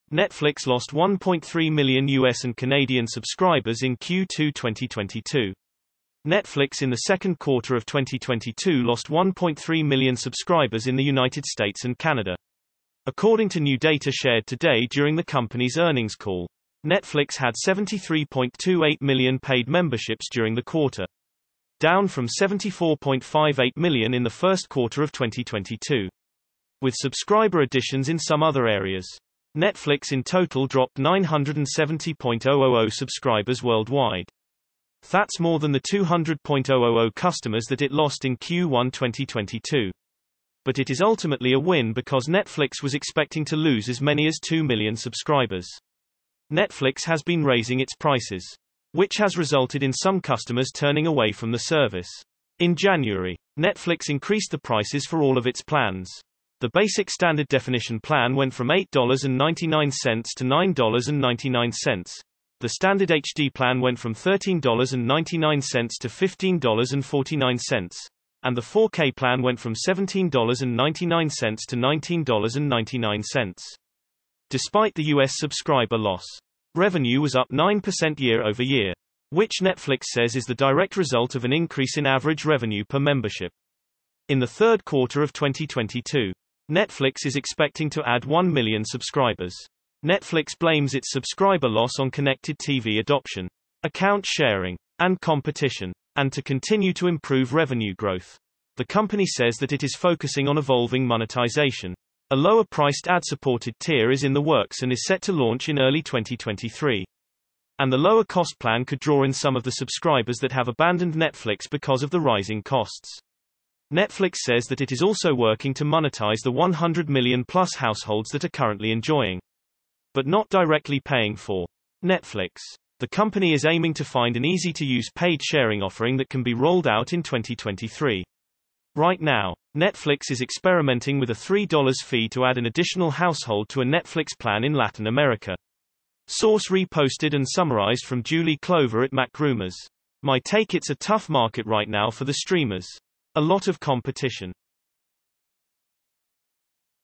Netflix Lost 1 3 Million U S and Canadian Subscribers in Q2 2022
Netflix in the second quarter of 2022 lost 1.3 million subscribers in the United States and Canada, according to new data shared today during the company's earnings call. Netflix had 73.28 million paid memberships during the quarter, down from 74.58 million in the first quarter of 2022.

Source: Re-posted and Summarized from Juli Clover at macrumors.

My Take: Its a tough market right now for the streamers. A lot of competition.